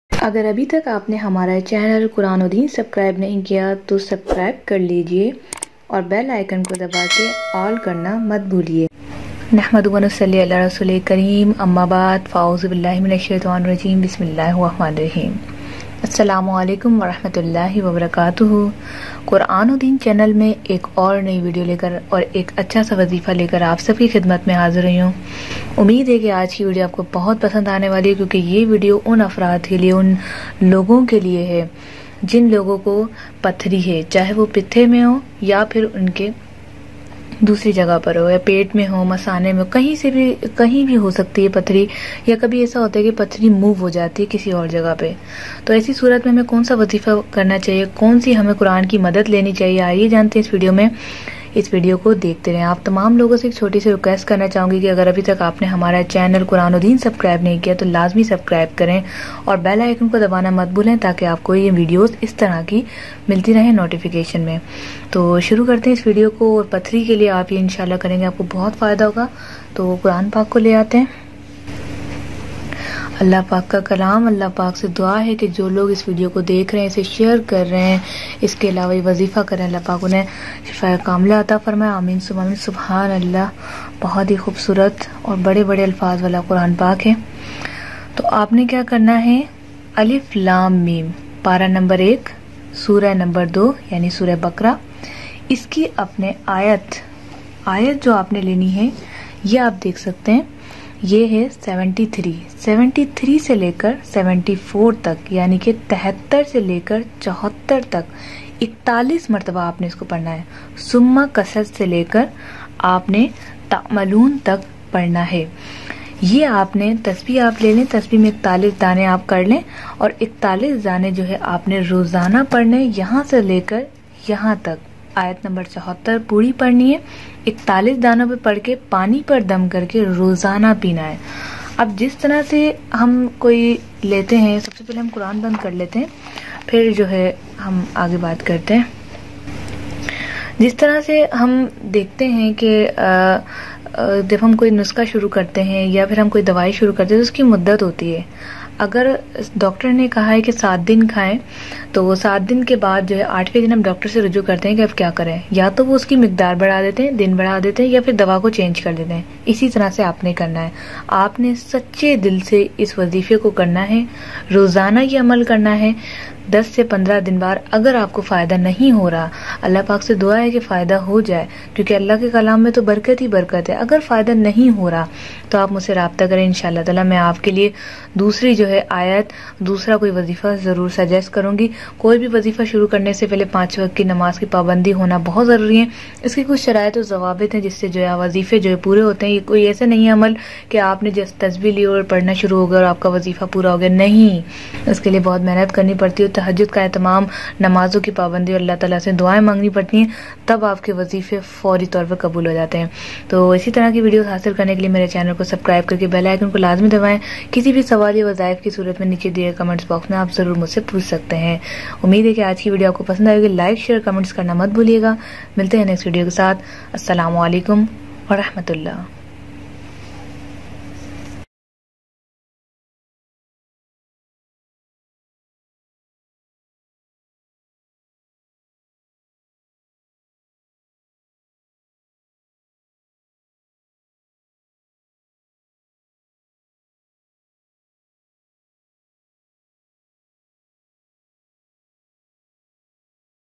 Pathri Ka Wazifa Quran Se - Belly Stone Treatment From Quran E Pak
Aj ki is video main mene bataya hai Pathri Ka Wazifa Quran Se - Belly Stone Treatment From Quran E Pak

شکریہ

Har Waqt Sardi Lagny Ka Ilaj - Kamzori Ka Ilaj - Khon Ki Kami Hona - Garmi Main Bhi Sardi Lagna